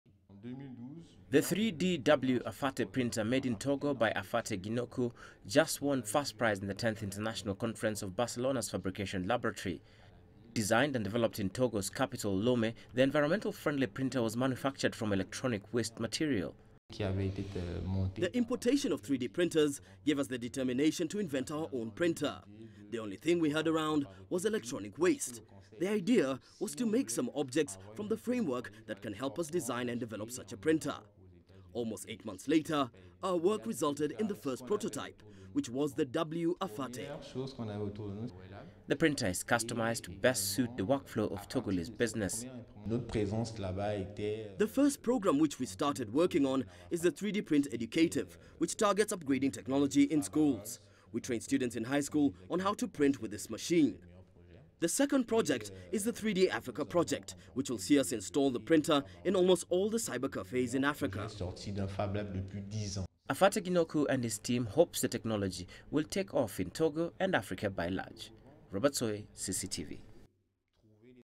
Togolese Inventor Wins Award For Technology Innovation
The 3D W.AFATE printer made in Togo by Afate Gnikou just won 1st prize in the 10th International conference of Barcelona's Fabrication Laboratory. Designed and developed in Togo's capital, Lome the environmental friendly printer was manufactured from electronic waste material